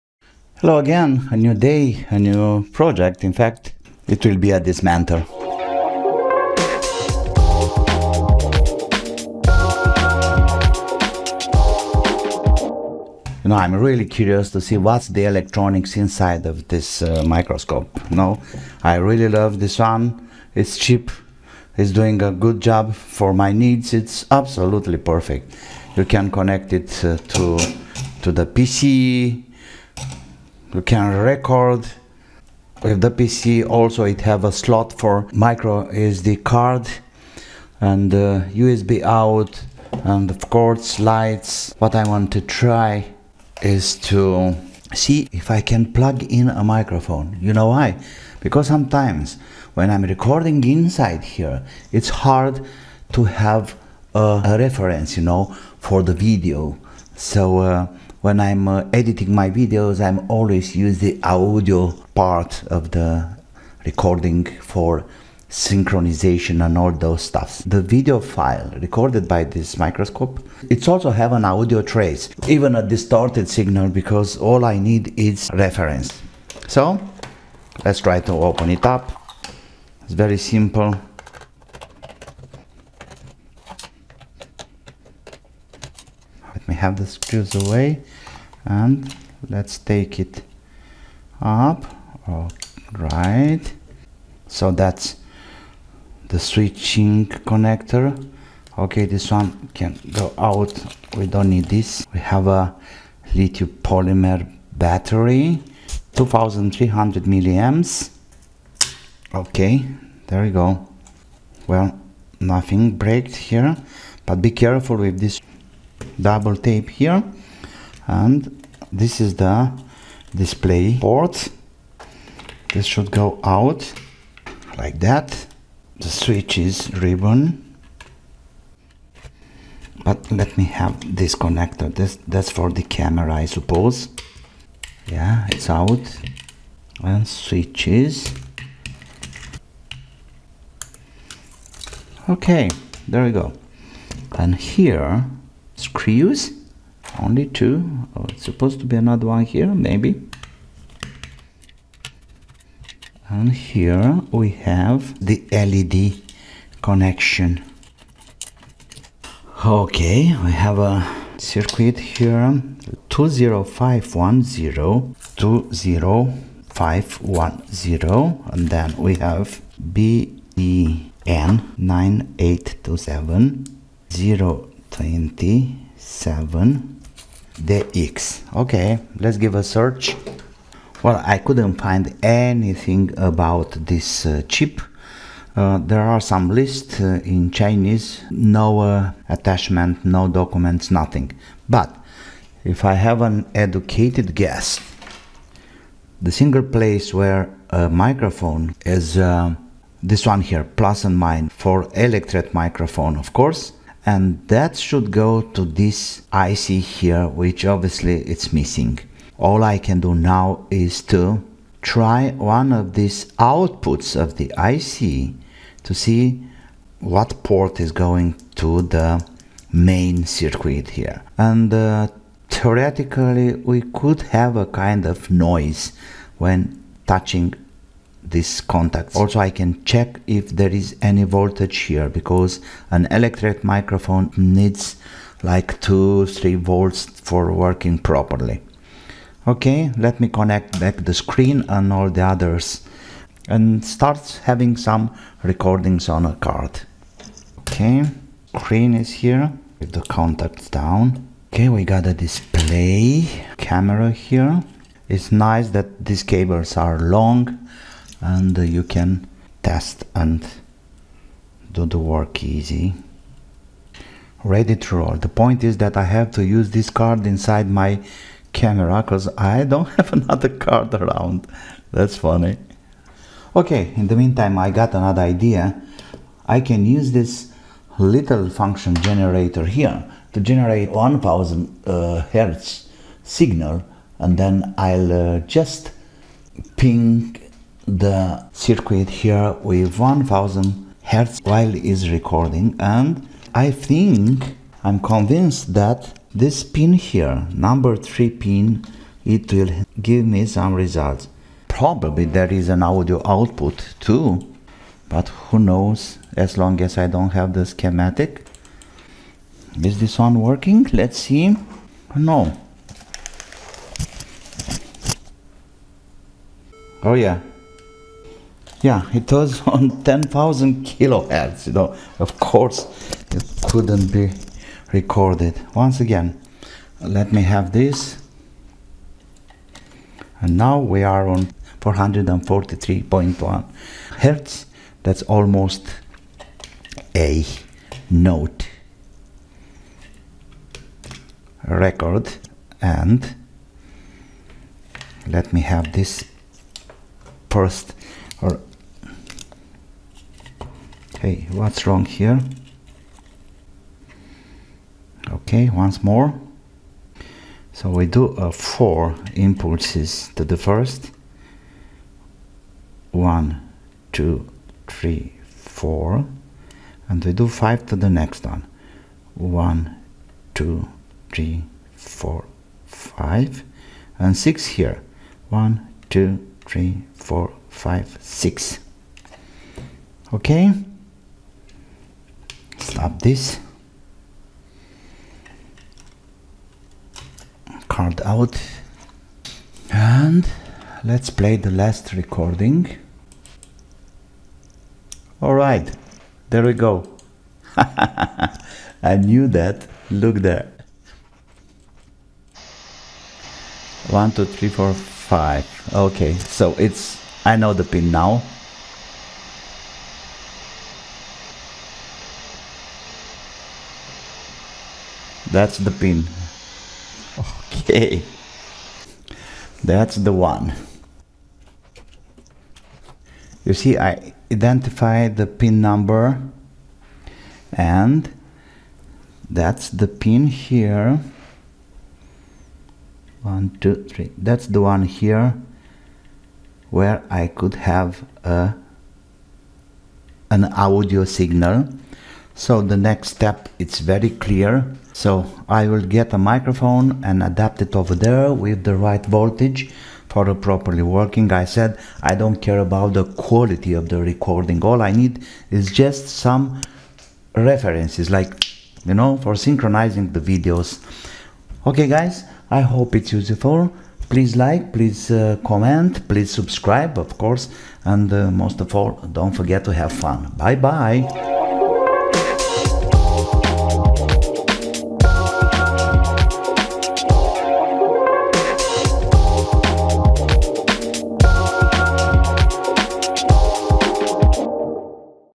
Microscope dismantle microphone MOD
Mustool G600 dismantle and search for an audio input for further microphone MOD